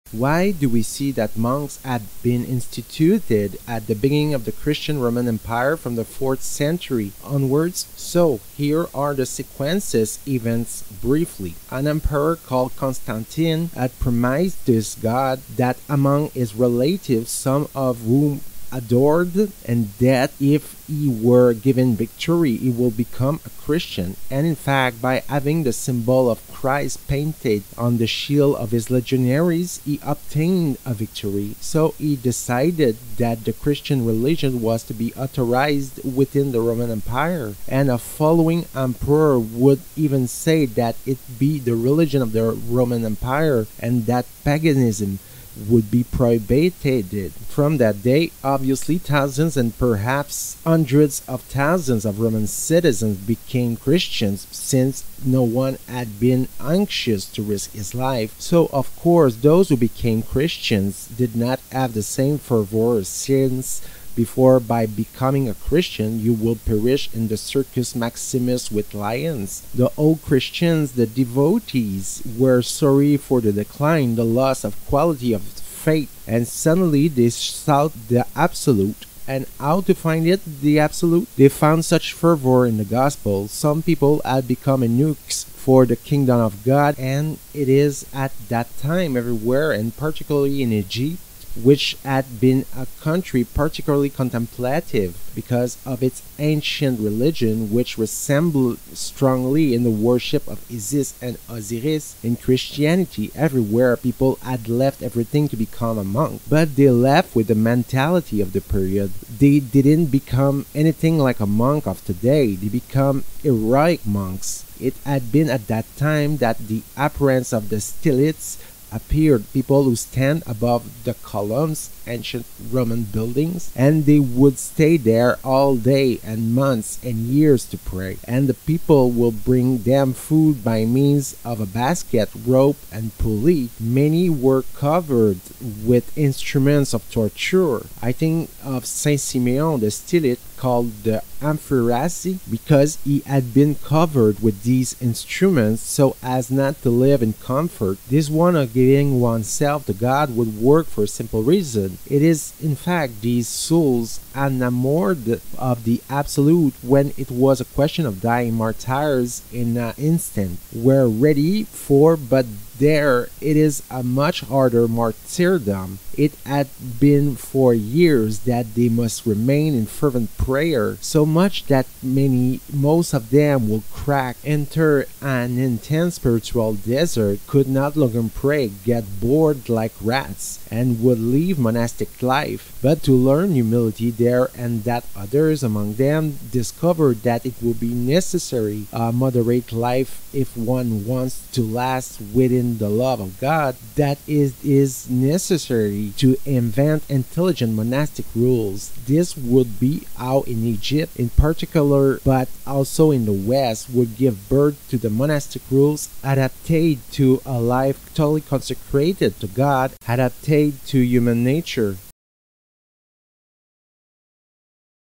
Acts of the Apostles 17- Why the institution of monks at the beginning of the Christian Empire?
English Translation of Actes des Apôtres 17- Pourquoi l’institution des moines au début de l’Empire chrétien ? (3 mn) (3 mins) Voice over by Sébastien Bédard
Why do we see that monks have been instituted at the beginning of the Christian Roman Empire, from the 4th century onwards?
So here are the sequences events briefly. An emperor, called Constantine, had promised this God - that among his relatives, some of whom adored - that if He were given victory, he would become a Christian. And in fact, by having the symbol of Christ painted on the shield of his legionaries, he obtained a victory. So he decided that the Christian religion was to be authorised within the Roman Empire. And a following emperor would even say that it be the religion of the Roman Empire and that paganism would be prohibited. From that day, obviously, thousands and perhaps hundreds of thousands of Roman citizens become Christians since no one had been anxious to risk his life.
So, of course, those who became Christians did not have the same fervour since, before, by becoming a Christian, you would perish in the circus-maximus with lions. The old Christians, the devotees, were sorry for the decline, the loss of quality of faith and suddenly they sought the absolute. And how to find it, the absolute? They found such fervour in the Gospel. Some people had become eunuchs for the kingdom of God and it is at that time, everywhere, and particularly in Egypt - which had been a country particularly contemplative because of its ancient religion which resembled strongly, in the worship of Isis and Osiris, in Christianity everywhere, people had left everything to become a monk.
But they left with the mentality of the period: they didn't become anything like a monk of today; they become heroic monks. It had been at that time that the appearance of the stylites appeared: people who stand above the columns (ancient Roman buildings) and they would stay there all day and months and years to pray. And the people would bring them food by means of a basket, rope and pulley. Many were covered with instruments of torture: I think of Saint Simeon the stylite called the "enfuirasse" because he had been covered with these instruments, so as not to live in comfort.
This way of giving oneself to God would work for a simple reason: it is, in fact, these souls, enamored of the absolute, when it was a question of dying martyrs in an instant, were ready for that but there, it is a much harder martyrdom: it had been for years that they must remain in fervent prayer. So much that many - most of them - would crack, enter an intense spiritual desert, could no longer pray, got bored like rats, and would leave monastic life, but to learn humility there and that others, among them , discover that it would be necessary a moderate life if one wants to last within the love of God, that it is necessary to invent intelligent monastic rules. This would be how, in Egypt in particular but also in the West, would give birth to the monastic rules, adapted to a life totally consecrated to God, adapted to human nature.